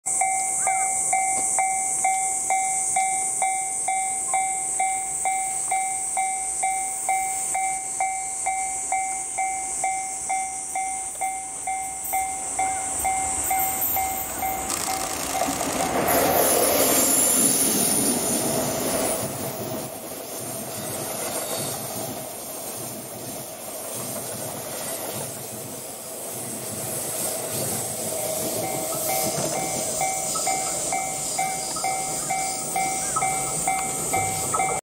【EF65‐2083牽引】東京メトロ17000系17183F 甲種輸送 早川～根府川通過【4K】
2021/7/17 JR東日本 東海道本線 早川～根府川にて

2021年7月16～18日に徳庵→綾瀬にて#東京メトロ 17000系17183Fの甲種輸送が行われました。吹田タからの牽引はEF65-2083でした。